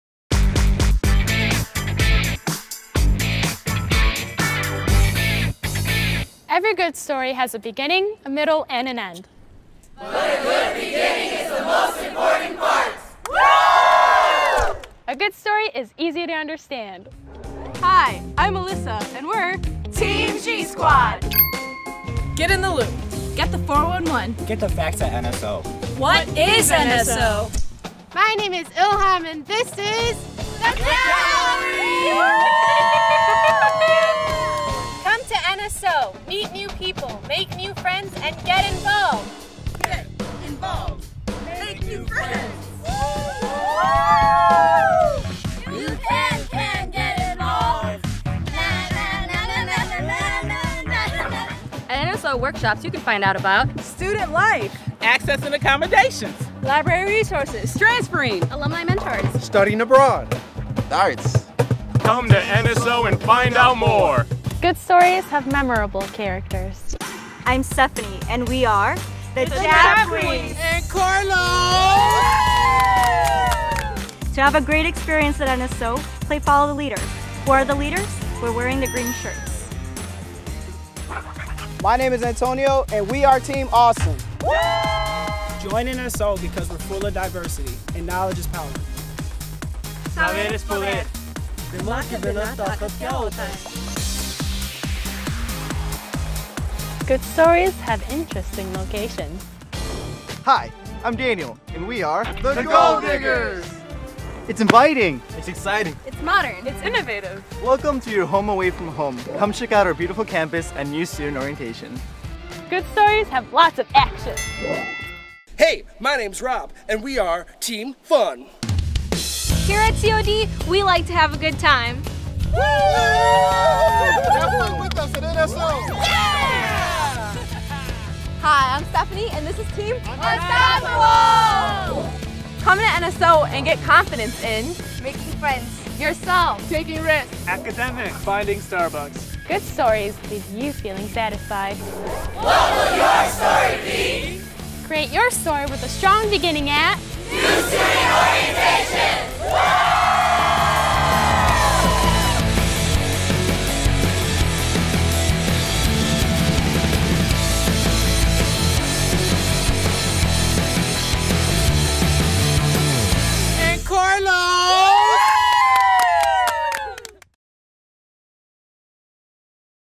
College of DuPage: 2015 New Student Orientation Welcome Video
What will your story be?  Kick off the new school year at New Student Orientation 2015!
Main campus in Glen Ellyn at the McAninch Arts Center (MAC).  Please choose one of the following dates:

Enjoy a free lunch social and have fun while meeting students, faculty and staff.